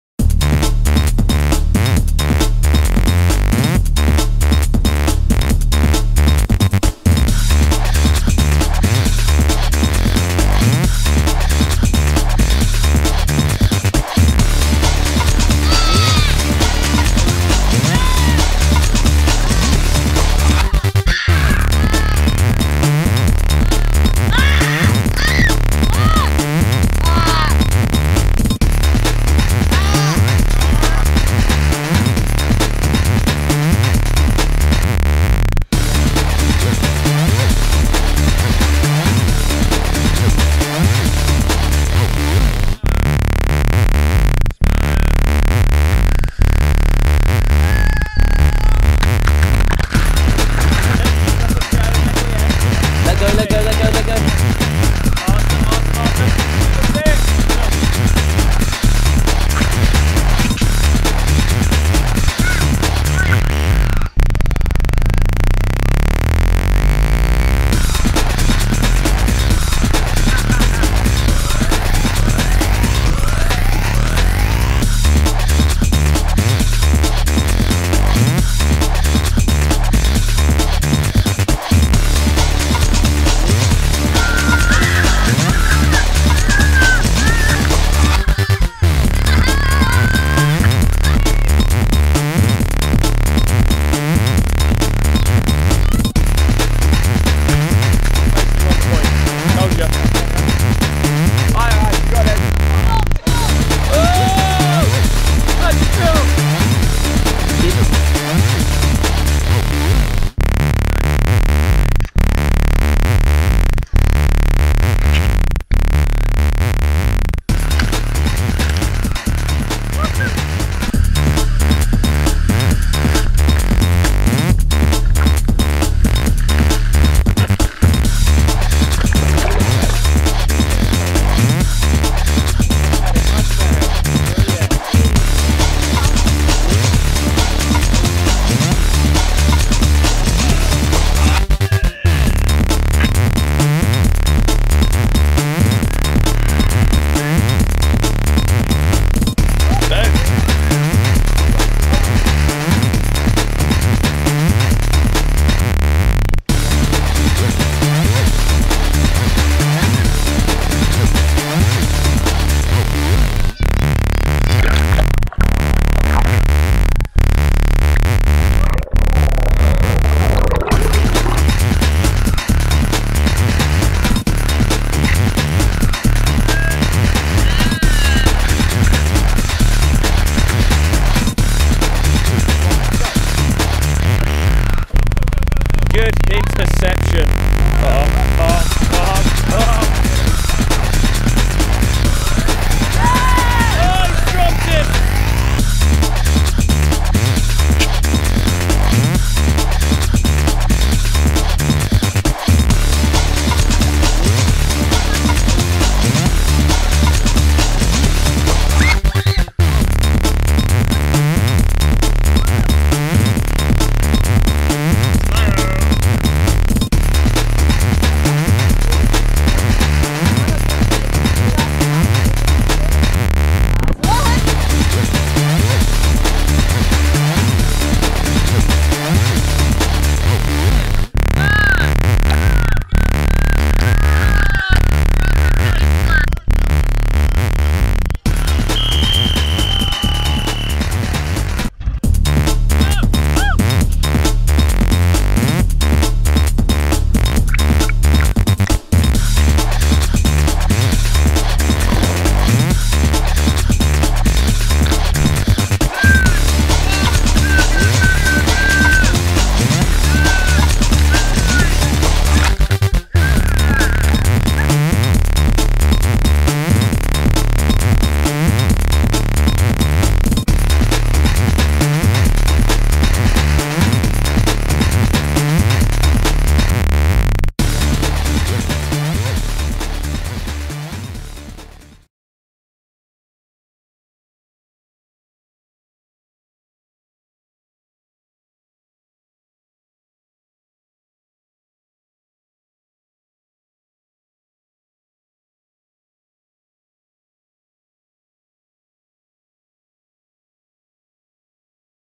Epic Spanish Watersports Adventure with Bridge of Don Academy ☀😎 | Voyager School Travel
Bridge of Don Academy have the best week of their lives in Tossa De Mar (Spain) with Voyager School Travel!